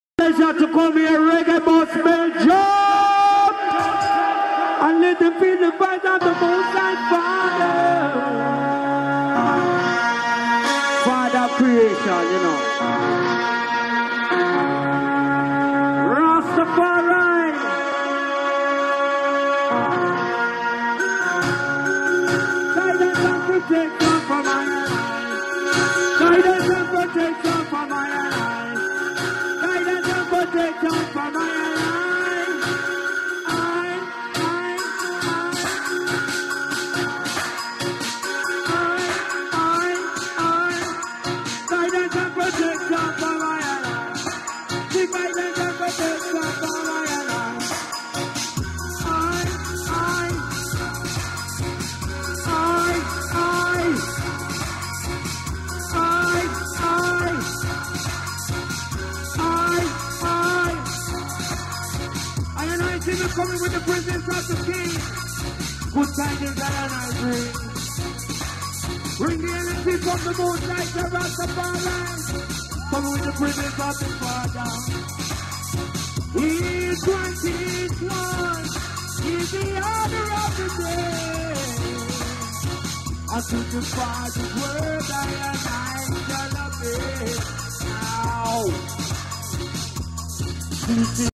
Aba Shanti - Reggaebus Festival 2012
Aba closing out his set...Ruff!